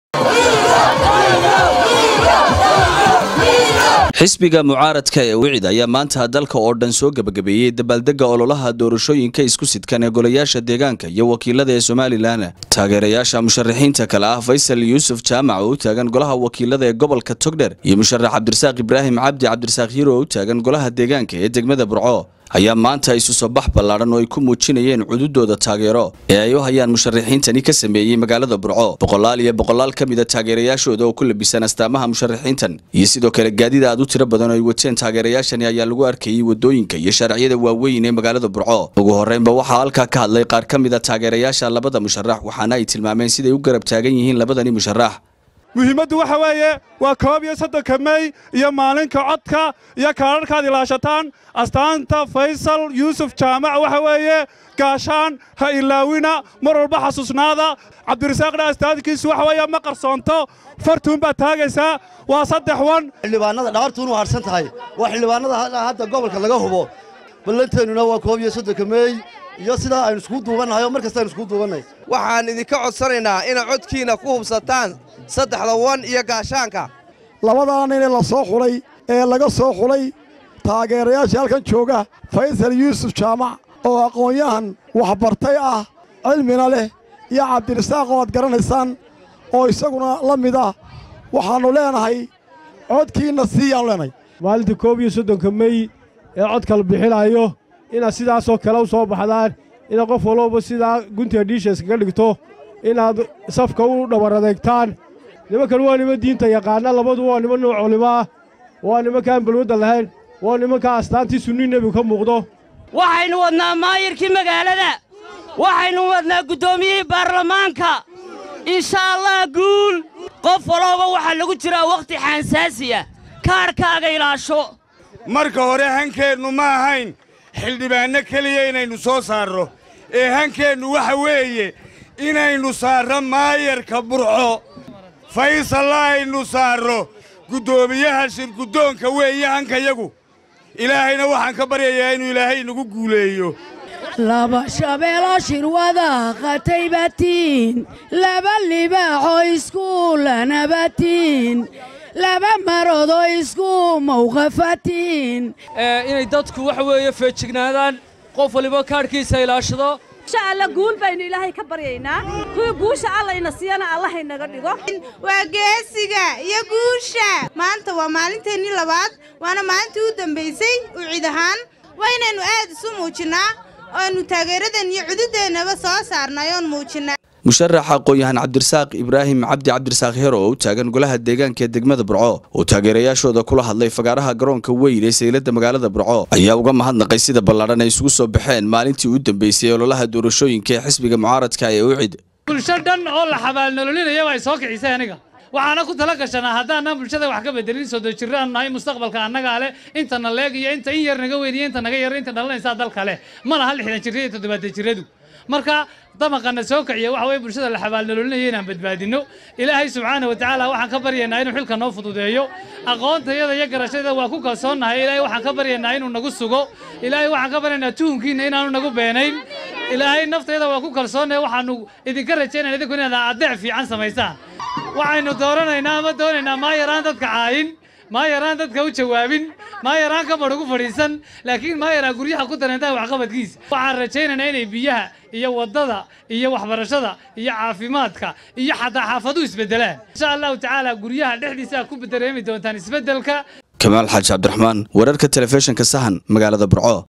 Muarashaxinta Faysal Yuusuf Jamaac Iyo C/qani Ibraahin Heero Ayaa Wac Daro Ka Dhigay Magalada Burco.
Muarashaxiinta Faysal Yuusuf Jamaac Iyo C/qani Ibraahin Heero Ayaa Wac Daro Ka Dhigay Magaladda Burco.